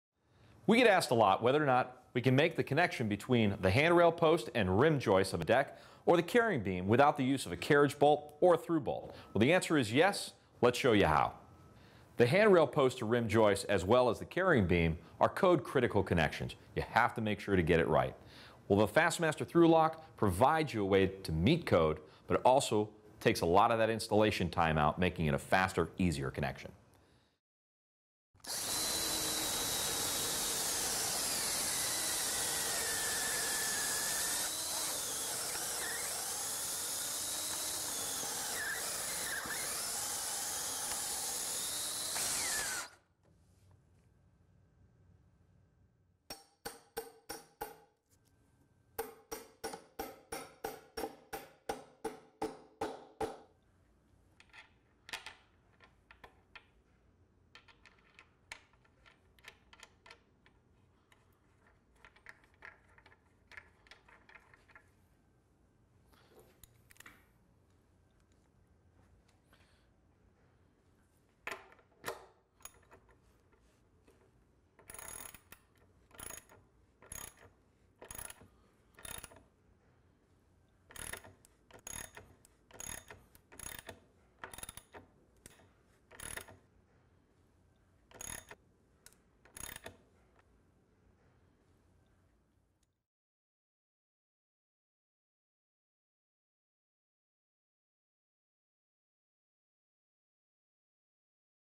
ThruLOK® at The Home Depot
ThruLOK® is a secure way to attach deck posts and carrying beams. and is much easier to install than a traditional carriage bolt, nut and washer. The traditional installation method requires a drill, drill bit, hammer, and wrench for install.  ThruLok®, however, only requires a high torque variable speed 1/2" drill. The ThruLOK® system combines the speed of a LOK product with the strength of a through-bolted connection in two decking applications: Single 2" x 4" board to a 4" x 4" decking post (6 1⁄4"), and two 2" x 4" boards to a 4" x 4"  decking post (8"). ThruLOK® works on all types of pressure treated wood.

(6 1/4") (8")